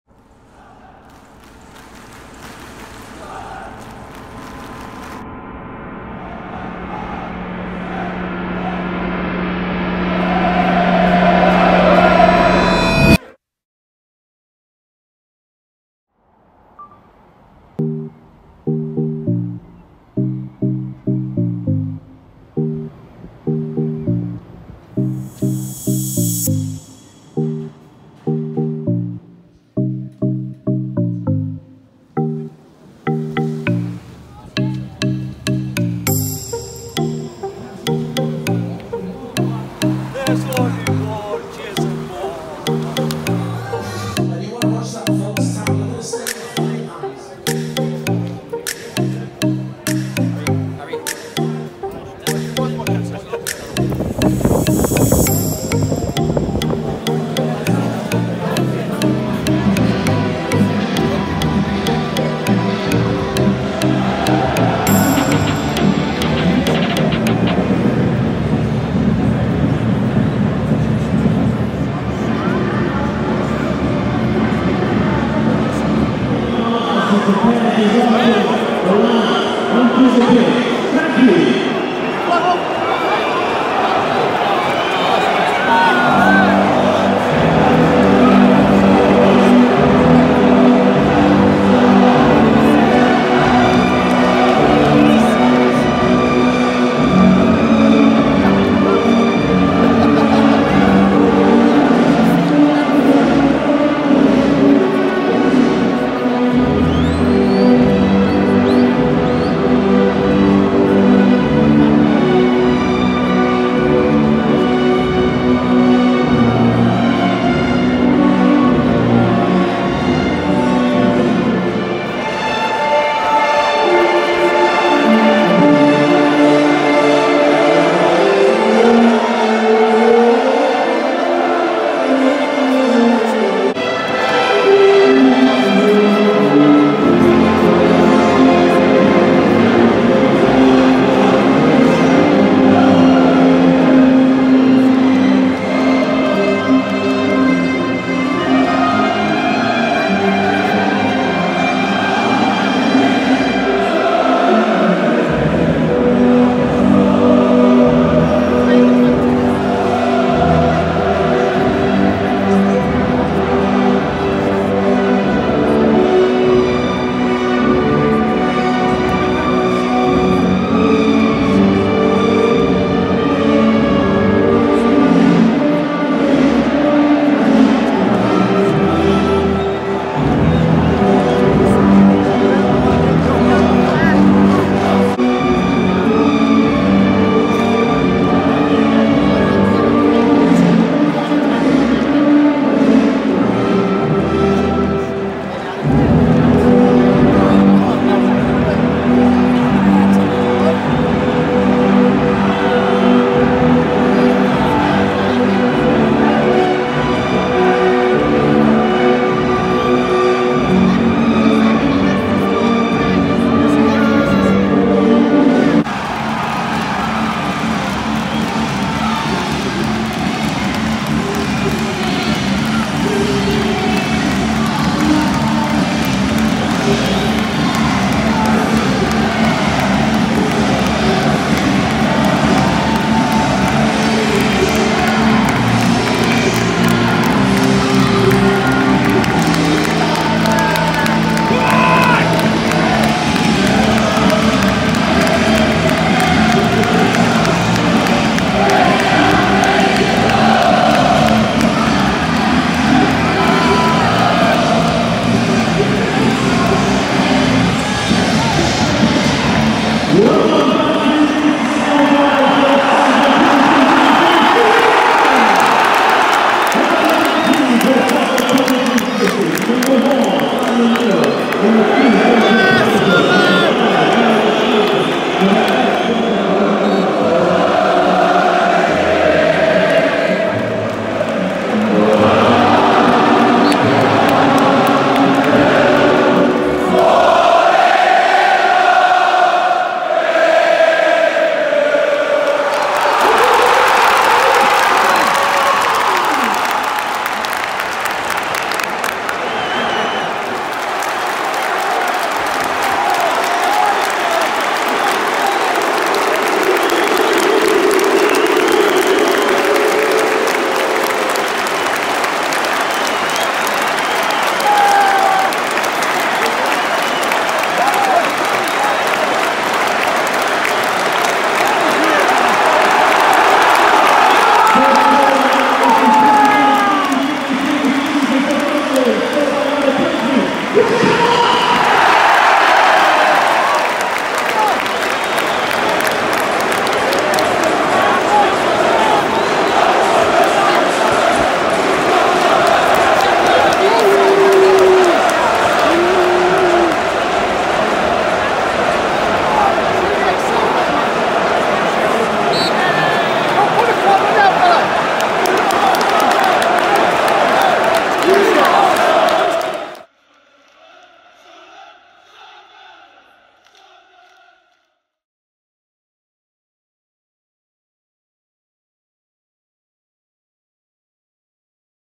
Sunderland 1-1 Coventry City - 31/07/22
Sunderland kick off the 22/23 EFL Championship season with a 1-1 draw against Coventry at the Stadium Of Light!

This video, like all of my away day videos, is purely intended to show a short snapshot of the day out. So no goals etc are featured. Its all about the fans.